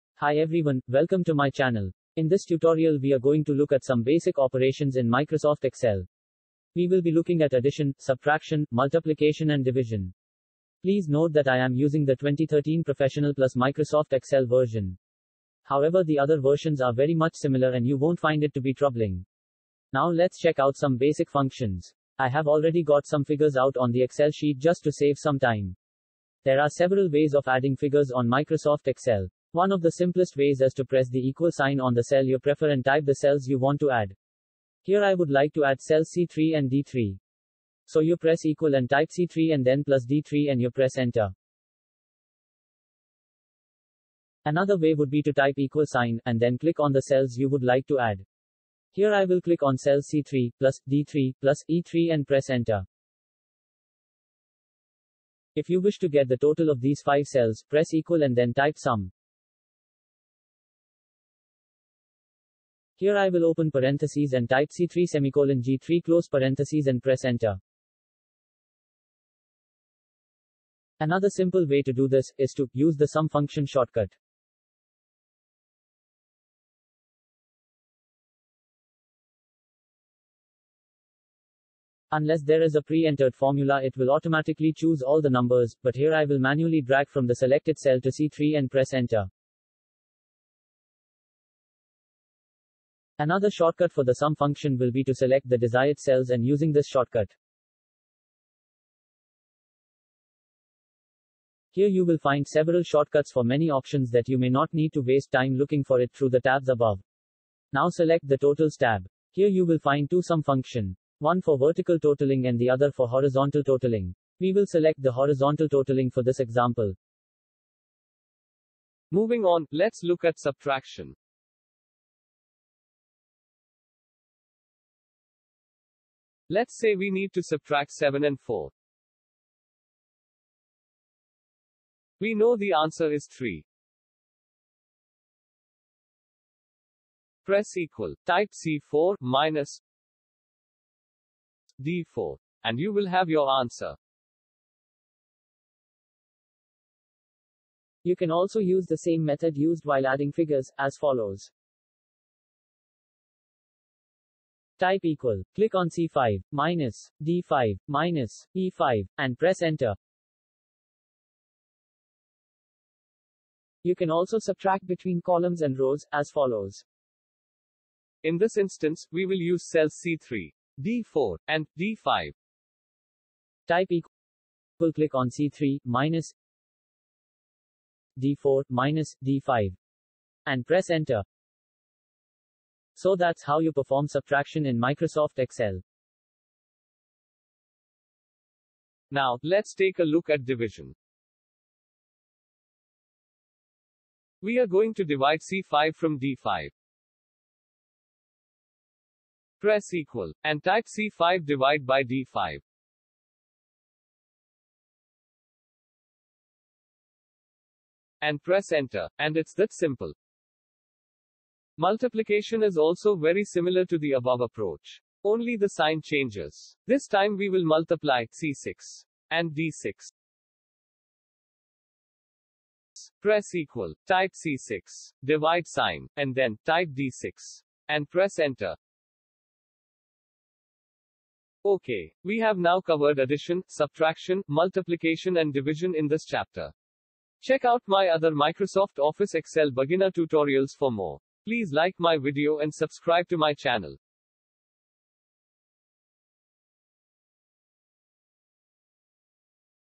Beginner's Guide to Microsoft Excel - How to Add, Subtract, Multiply and Divide in Excel
This video is a guide for the beginners to Microsoft excel. This tutorial covers in-depth lessons on how to use addition, subtraction, multiplication and division.